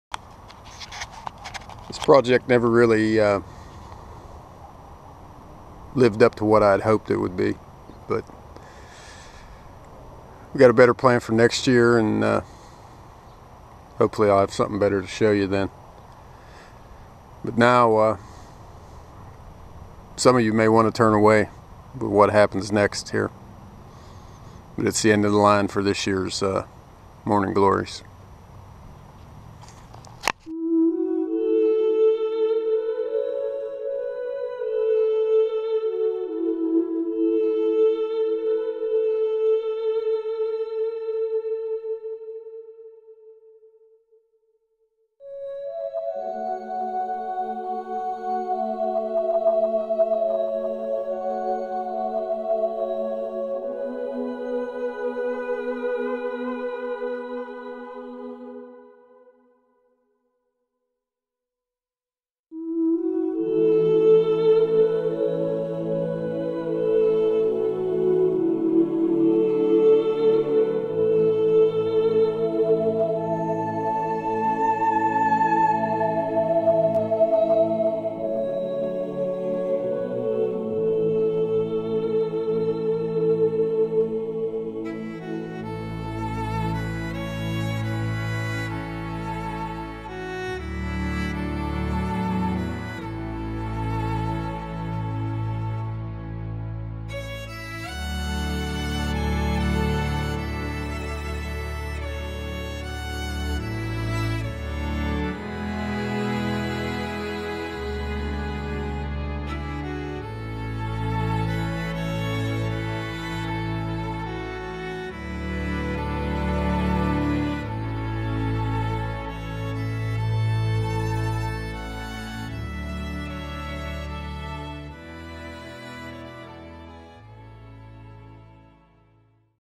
Sad End For Flowers For Shade Project
The Flowers for Shade Project has come to a sad end. But I learned a lot and there is hope for next spring. If everything goes as planned that wall will be covered with morning glory's by the end of May.

And when you get a chance check out the other HWDsouth channels